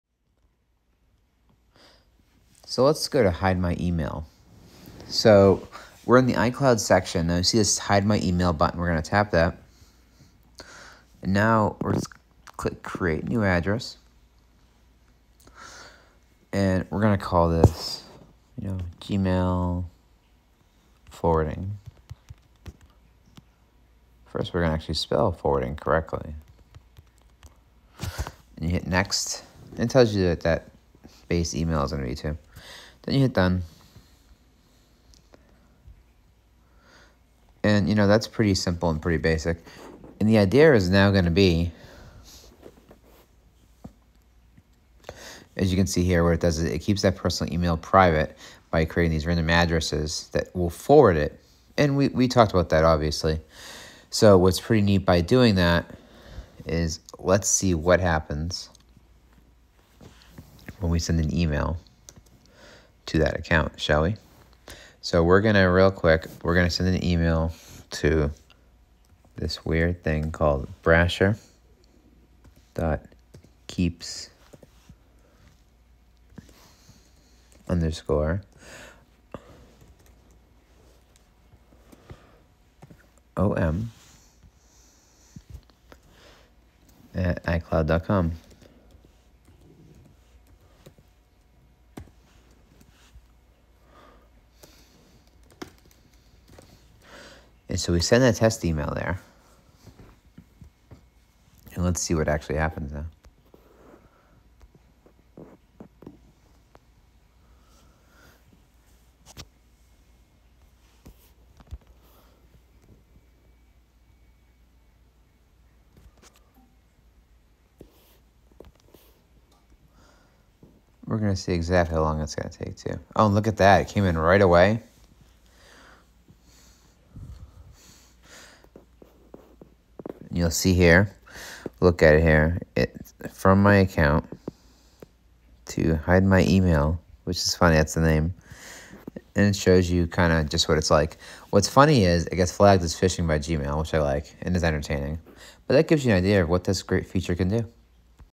iOS 15 Hide my Email Demo
A short demo detailing the new feature in iOS15 called "Hide my Email" to anonymize email.